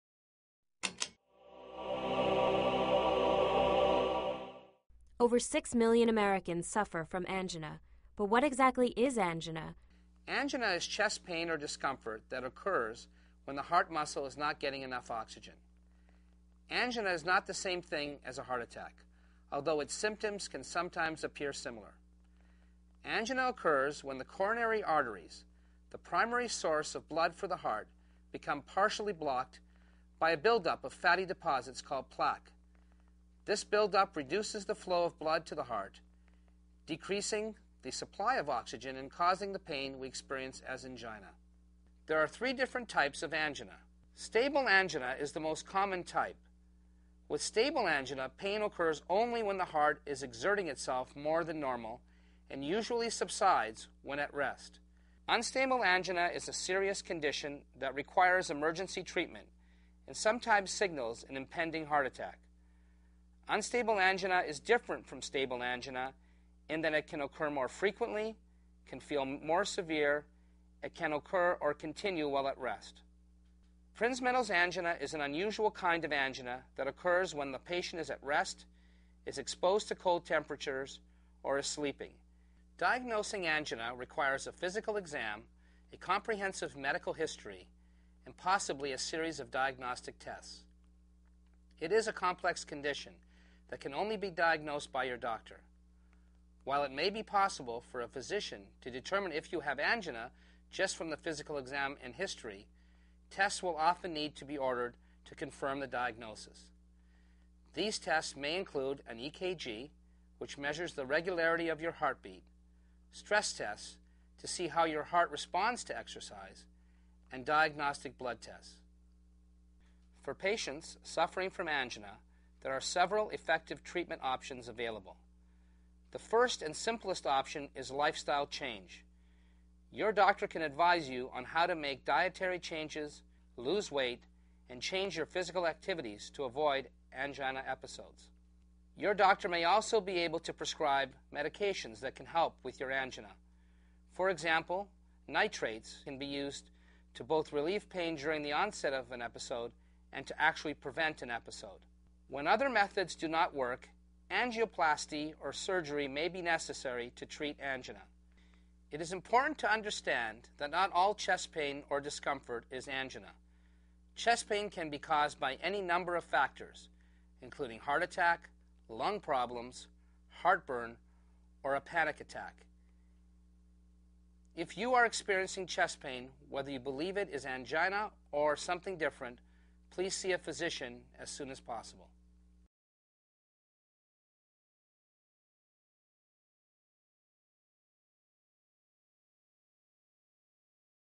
What is Angina (Heart Basics #3)
Over 6 million Americans suffer from the angina, yet many people have never even heard the term.